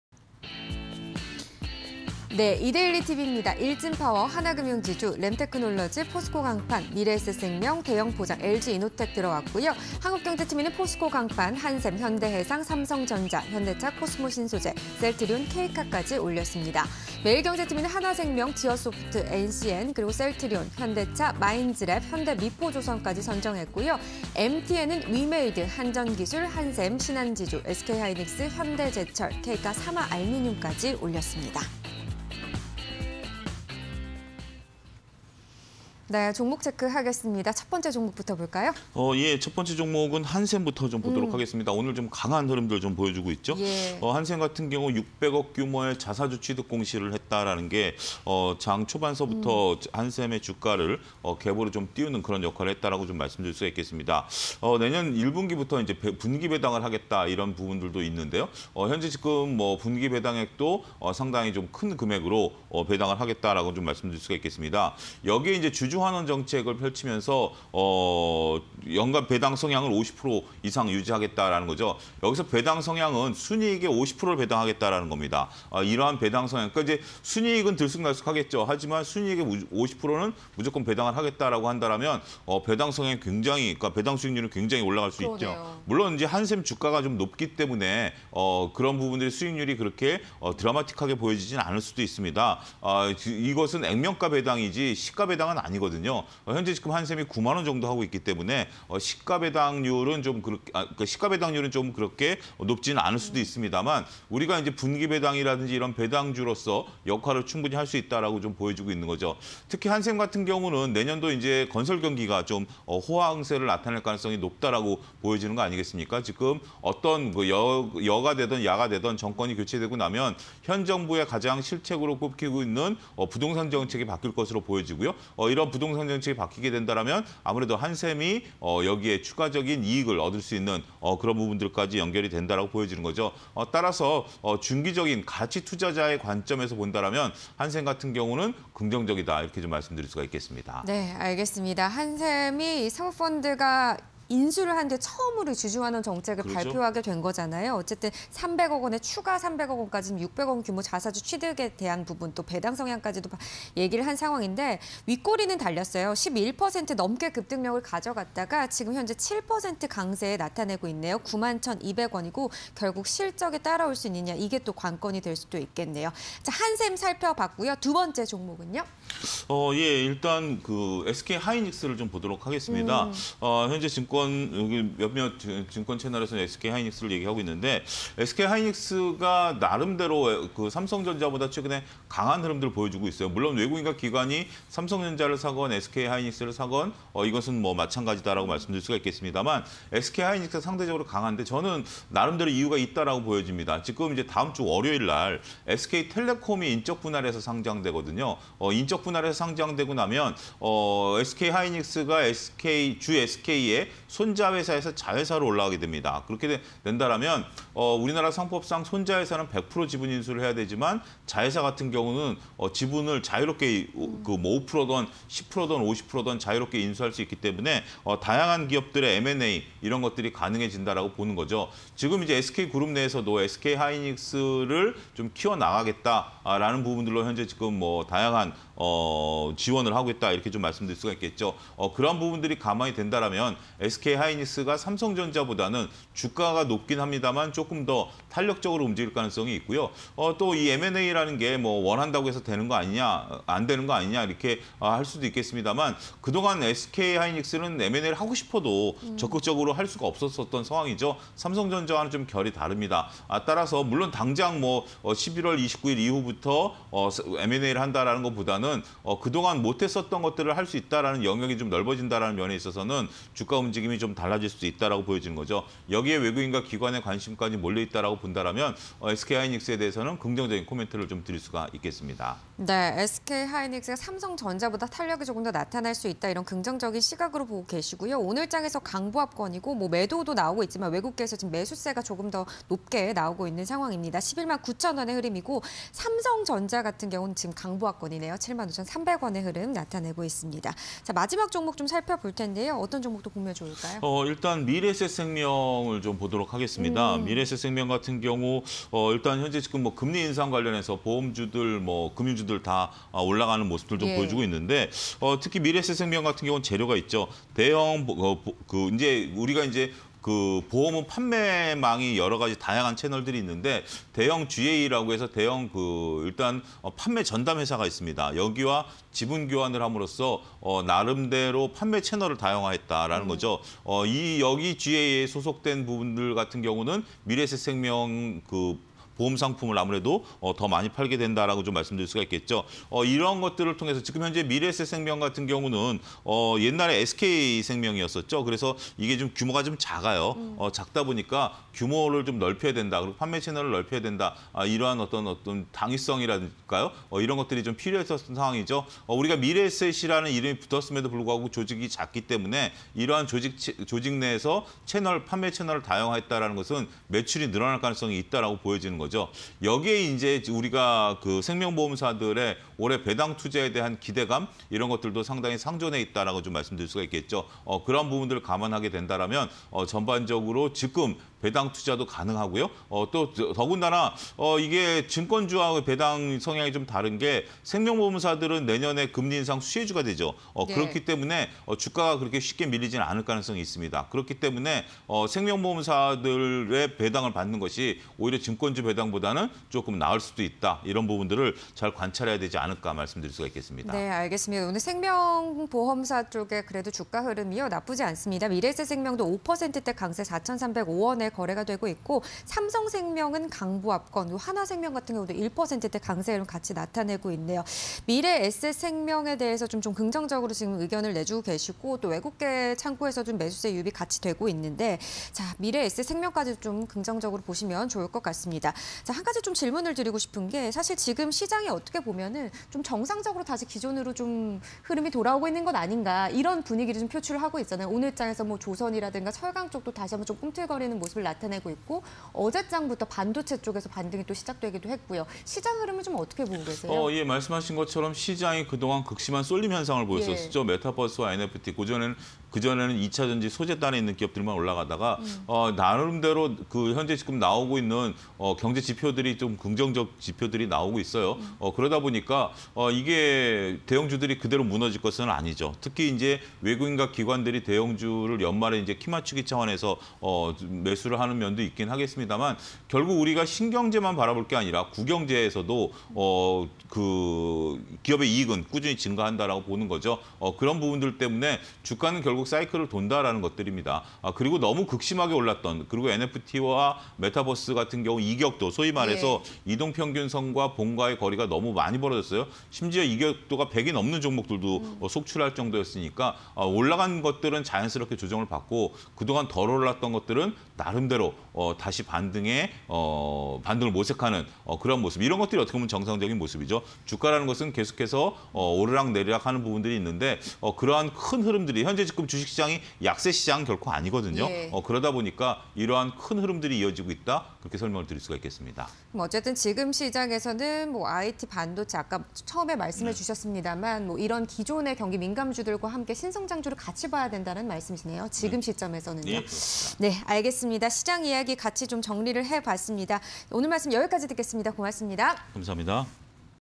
한샘, SK하이닉스, 미래에셋생명_특징주 공장 (20211123)
한샘(009240)
- 600억 규모 자사주 취득 공시
- 내년 1분기부터 '분기 배당' 실시
- 중장기 주주환원 정책 강화 움직임
- 2022년 건설 경기 호황 기대

- SK텔레콤, 29일 변경상장 및 재상장
- 테크·플랫폼 영역의 다양한 M&A 기대감
- 외국인 및 기관 관심 확대

미래에셋생명(085620)
- 대형 법인보험대리점(GA)와 지분 투자 계약
- 판매 채널 다양화 기대
- 배당 투자 관심